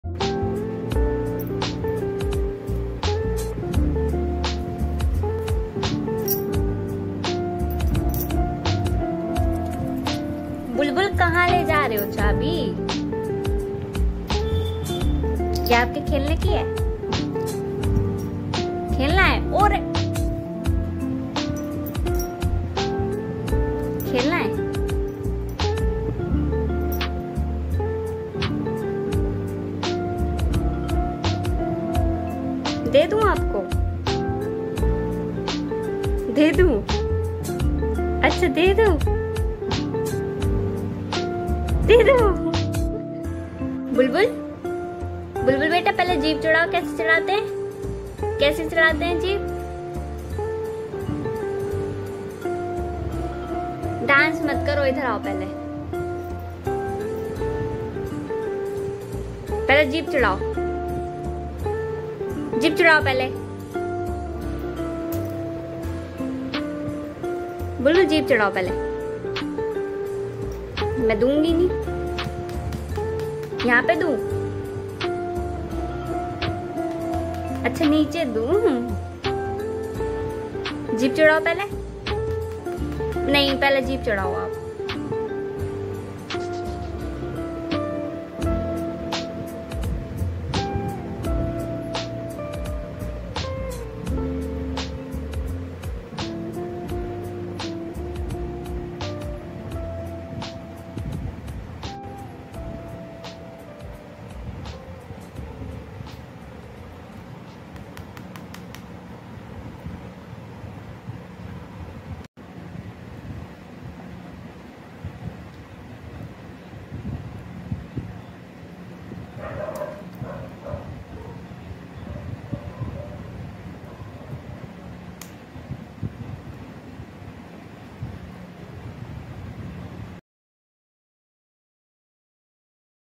Play With My Parrot 🦜 bulbul ❤ | The Hommie girl || Aj masti kre bulbul ke sath
vlogs daily vlogs

aj khele bulbul ke sath

The hommie girl present